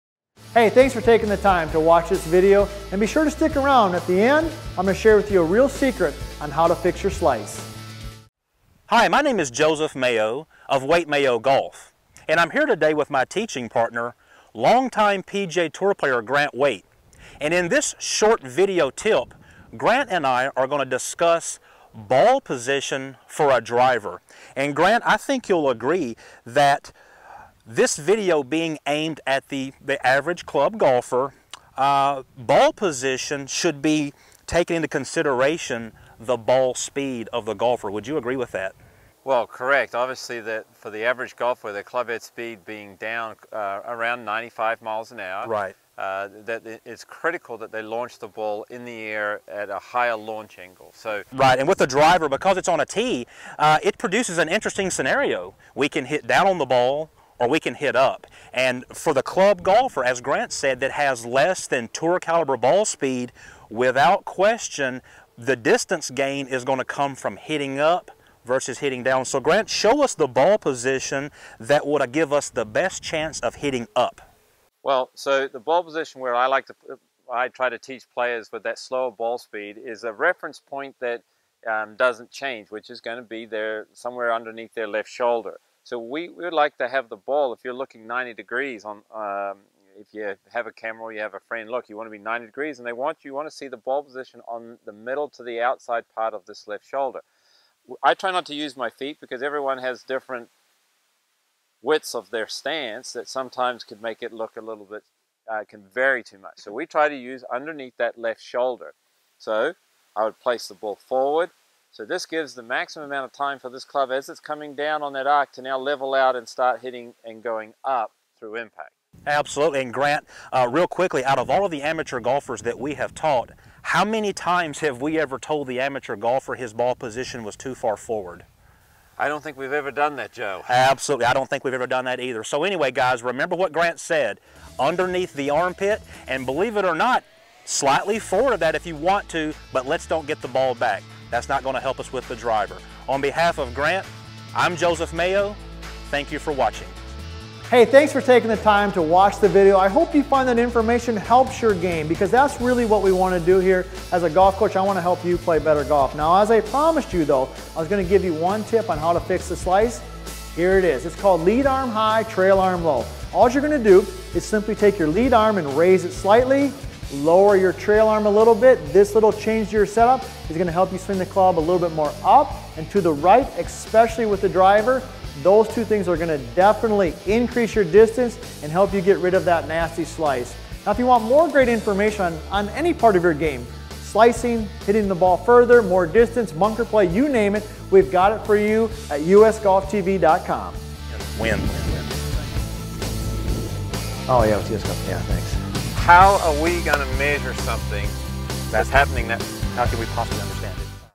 How Can Ball Position Add Yards to Your Driver?
When you’re hitting shots, how often do you consider the ball position?

You probably think the secret to hitting longer drives is the brand-new $400 driver with the speed grooves, secret slots, adjustable weights, and mega-fast face.

Either that, or you think you just need to figure out how to swing the club faster.

Maybe you’re the person who thinks, “all I need to do is get my swing to look like the ones on TV and I’ll start bombing tee shots.”

WHAT IF I TOLD YOU, A SIMPLE THING LIKE BALL POSITION CAN HAVE A DRASTIC INFLUENCE ON THE DISTANCE YOU HIT YOUR DRIVES?
In this video, PGA teaching professionals and industry leaders Joseph Mayo and Grant Waite talk about the importance of solid ball position.

DRIVER BALL POSITION
When looking back at this video, this dynamic duo dives into the importance of strong ball position when hitting drivers.

The average golfer swings the ball at 90 miles per hour. This is drastically different than what the average PGA Tour player swings their driver, which is typically in excess of 110 miles per hour.

Simple physics tells us amateur golfers need to do everything they can to give themselves optimum yardage.

Since the golf ball sits on a tee with the driver, this means a golfer can do one of two things:

They can either…

Hit up on the ball (positive Angle of Attack), or…
Hit down on the ball (negative Angle of Attack)

Now you’ll read a lot of blogs on the internet from people with various opinions on this topic. But Joseph Mayo makes an unequivocal statement to understanding this.

FIX YOUR SLICE TODAY!